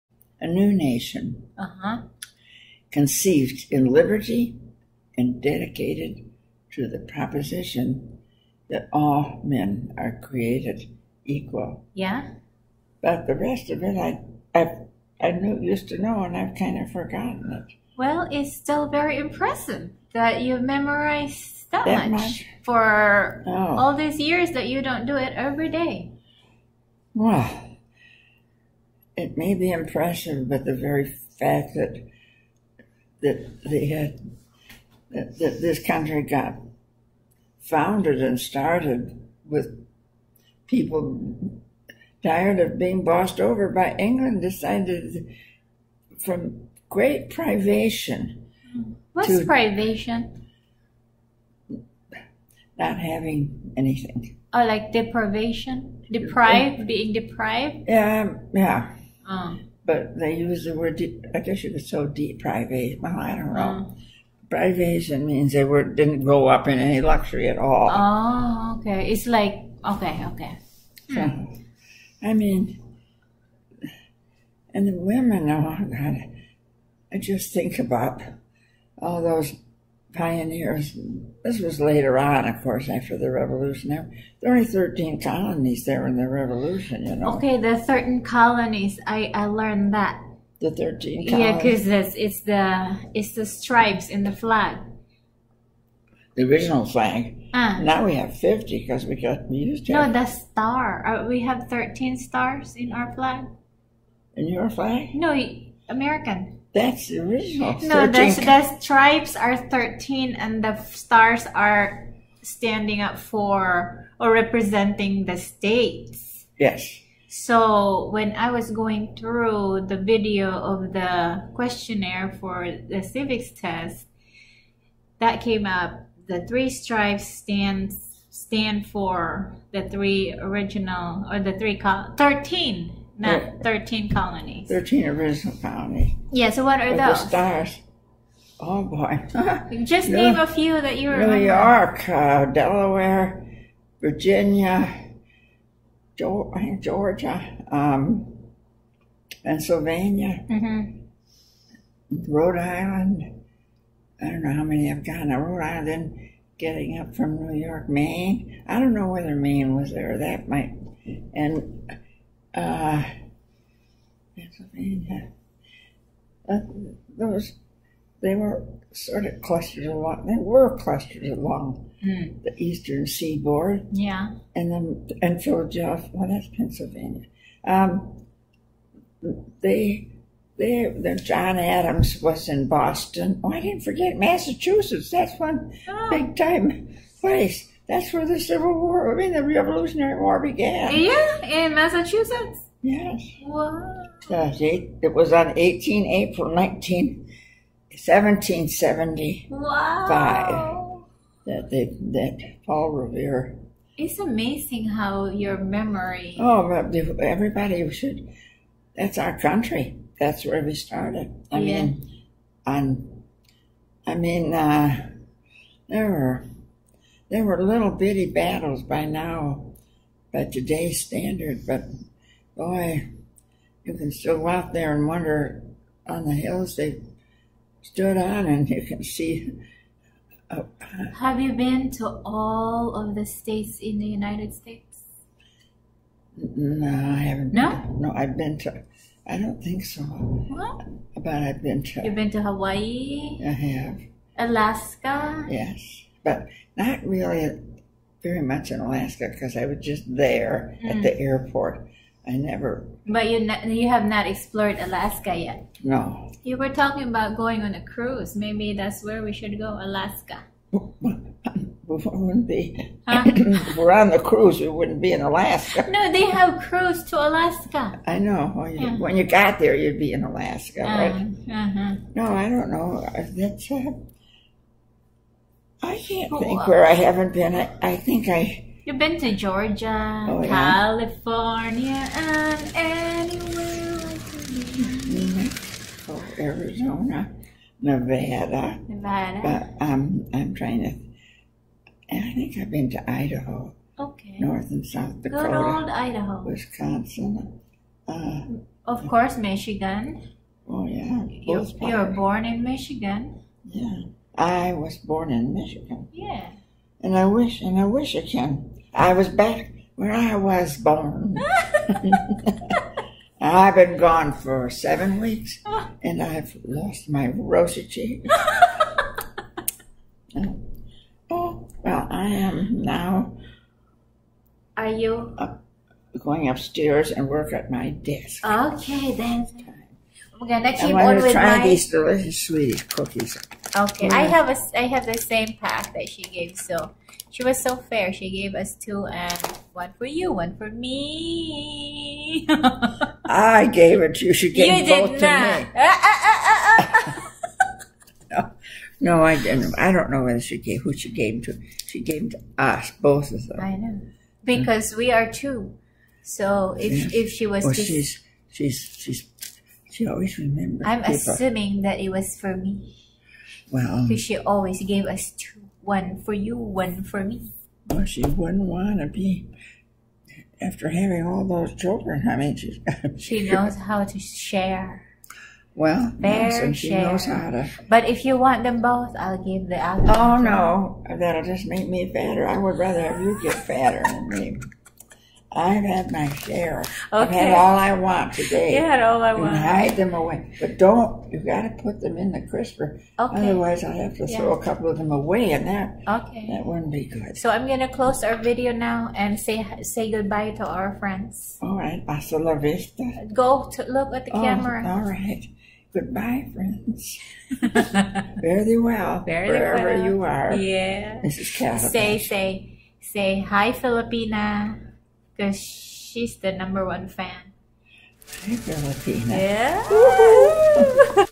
Amazing 94 year old grandma with impressive memory || Conversation with Lola
94-year old and caregiver conversation